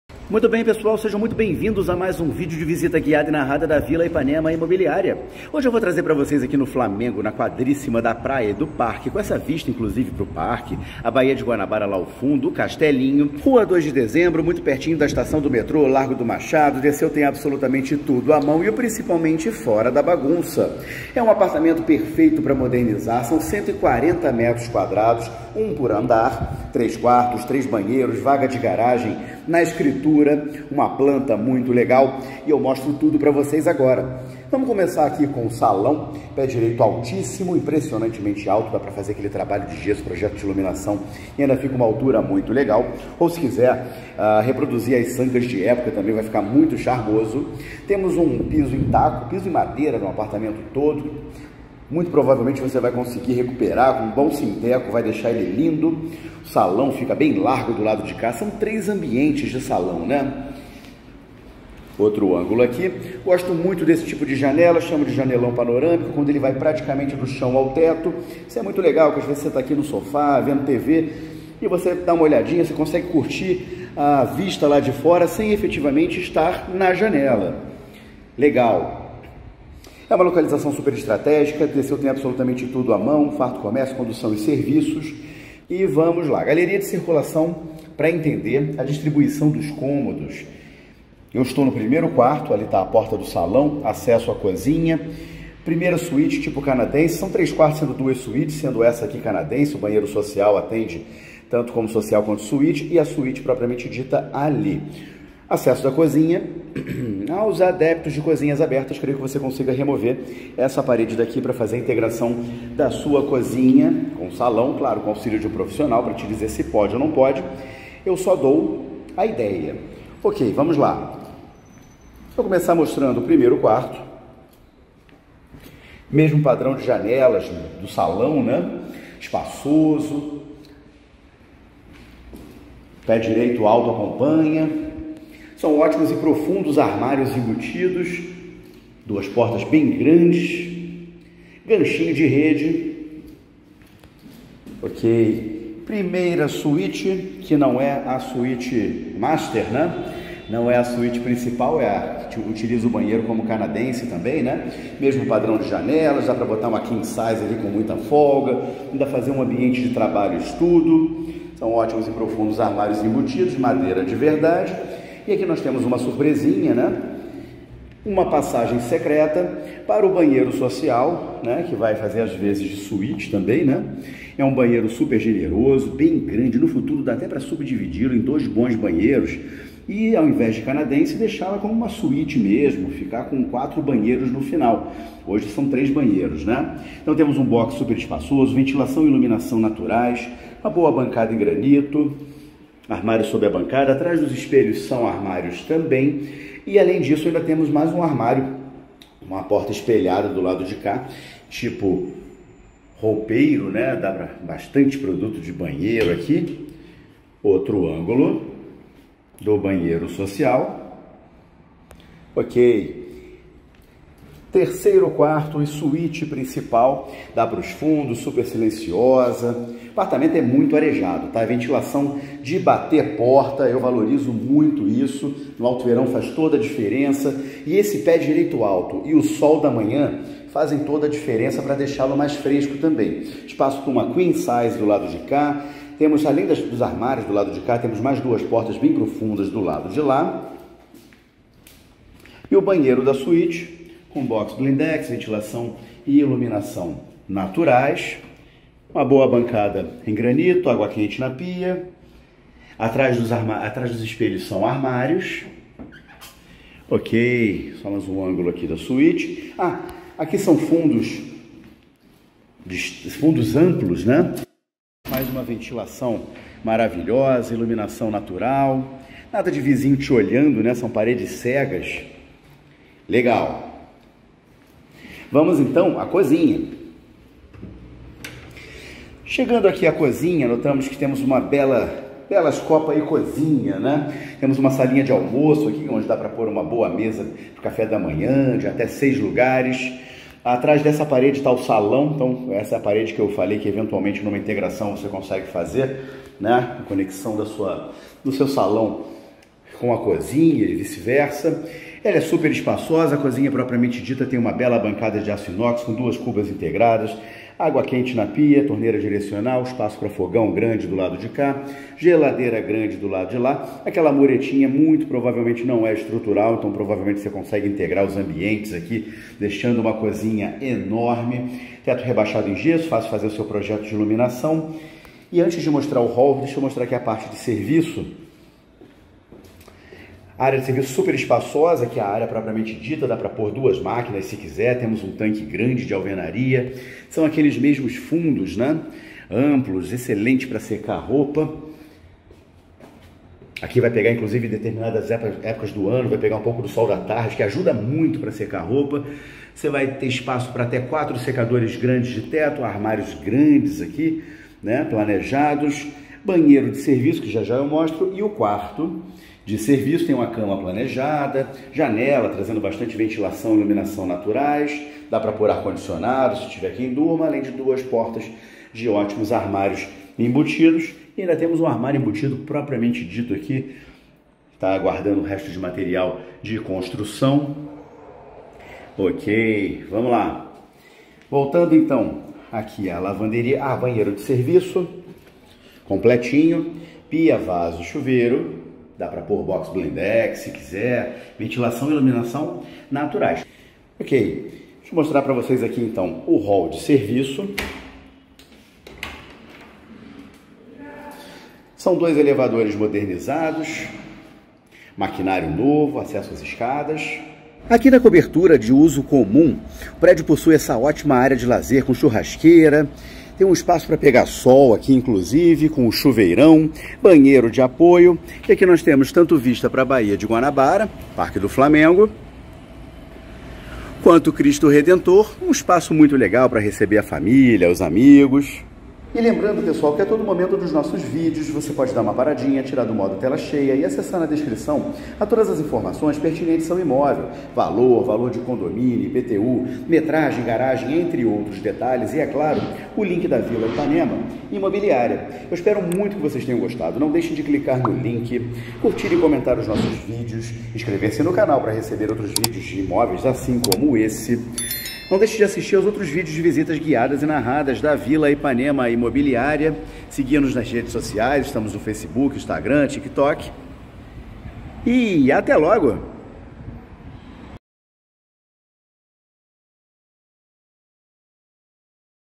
FLAMENGO I QUADRA DA PRAIA E PARQUE I 1 P ANDAR I 140M2 I 3 QUARTOS 2 SUÍTES I CHURRASQUEIRA I VAGA
VENDIDO!
Valor do imóvel: R$ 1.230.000,00
Condomínio: R$ 1.835,00
IPTU: R$ 248,00
Código: CAPTA1008

Quadra da praia/parque! Linda vista lateral para o castelinho!

1 por andar, sol da manhã, pé direito alto, silencioso e arejado!

Salão para 3 ambientes, ótima galeria de circulação, 3 quartos com ótimos armários embutidos sendo 2 suítes sendo 1 tipo canadense!

Banheiros social/canadense e suíte completos e ventilados.

Sala de almoço destacada da cozinha planejada, área de serviço espaçosa e ótimas dependências completas de empregados! Banheiro de serviço completo!

Cobertura de uso comum com churrasqueira, chuveirão e vistas para o cristo e baía.

Prédio recuado com jardins, charmosa portaria, câmeras de segurança e vaga de garagem na escritura!

Valores sujeitos a confirmação.

CJ: RJ - 008630/O